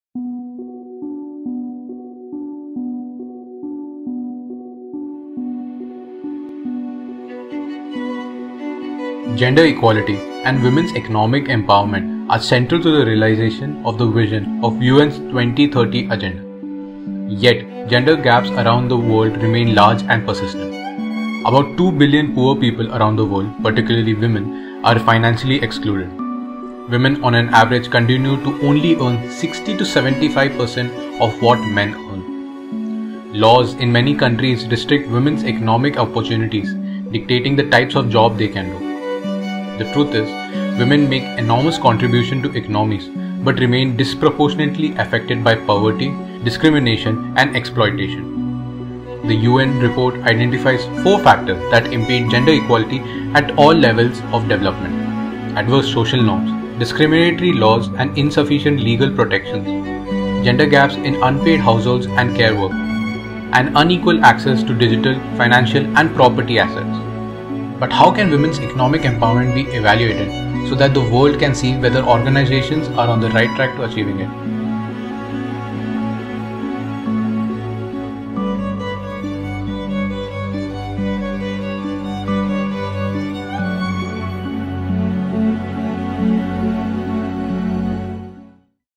Agenda Video: UNW
VIT Pune MUN is glad to release the agenda videos for the committees we'll be simulating at our 2021 Conference.

Presenting the agenda video for UNW (United Nations Women).
Agenda: Discussing Measures for Economic Empowerment of Women.